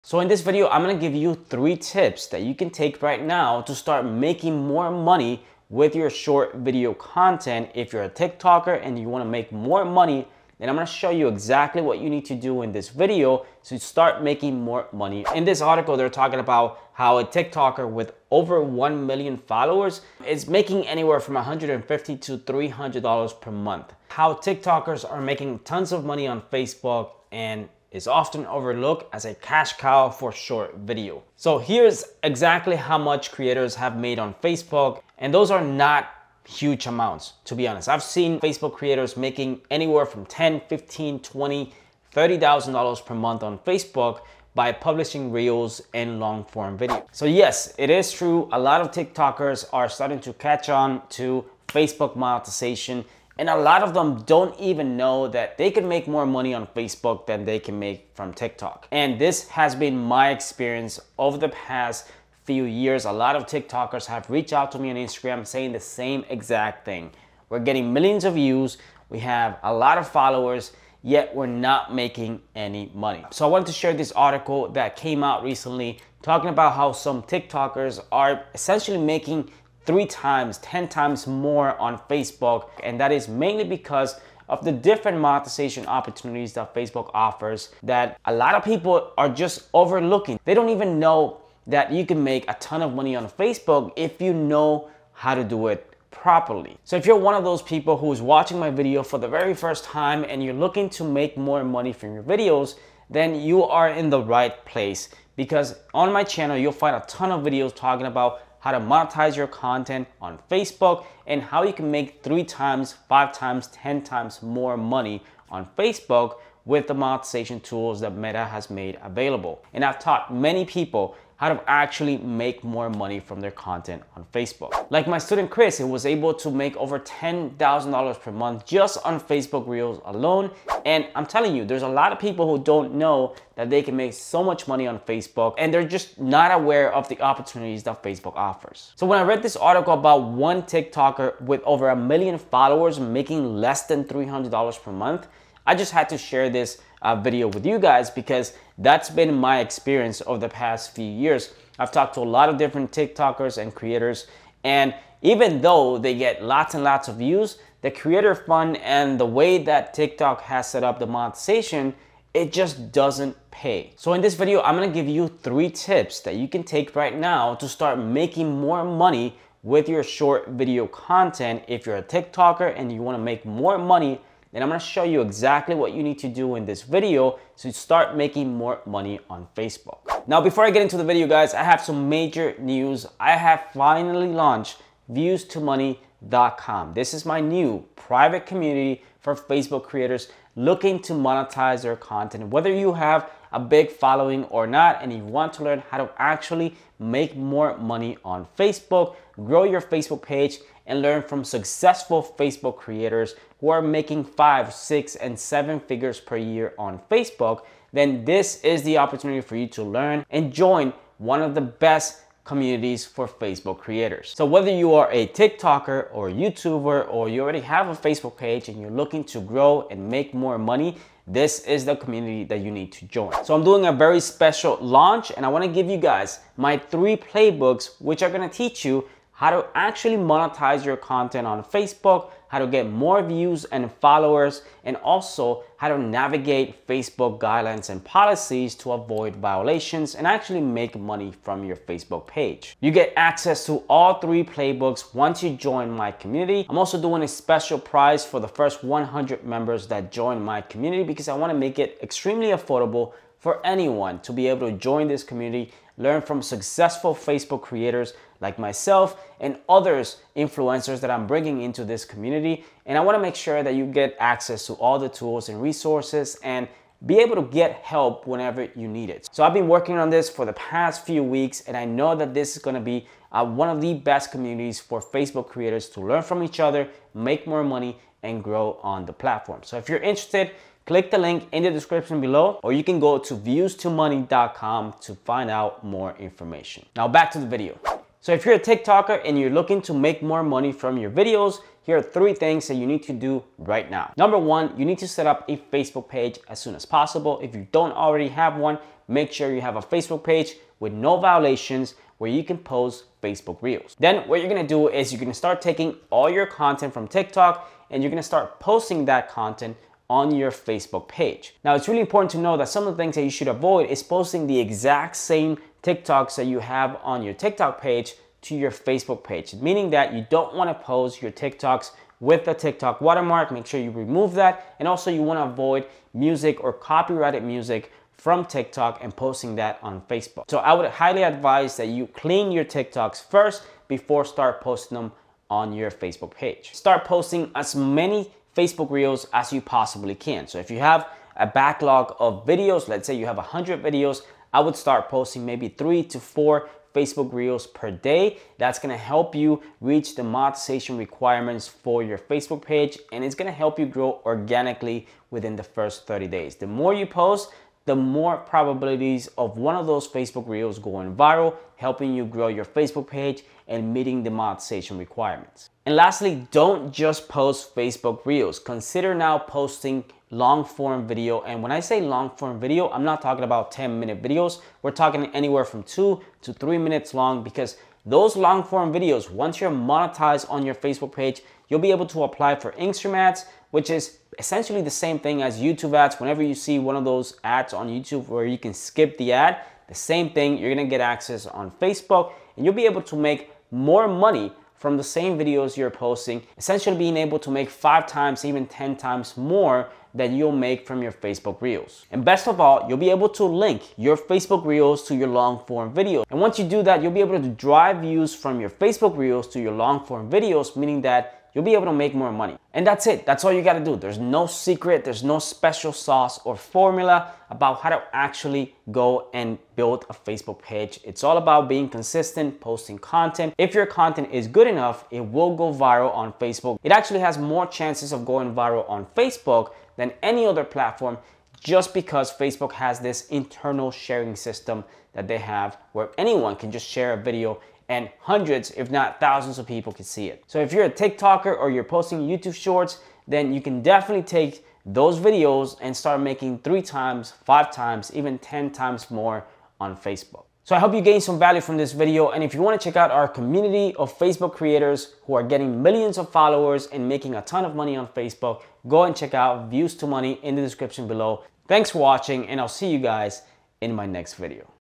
How To Earn 10X More Money Repurposing Your TikToks!
If you want to make money on TikTok then in this video I'll break down how to earn from your TikTok videos by repurposing them to Facebook.

For YouTubers & content creators looking to work 1-ON-1 with Lester!

Free training: How I made $23,197.10 in 90 days as a content creator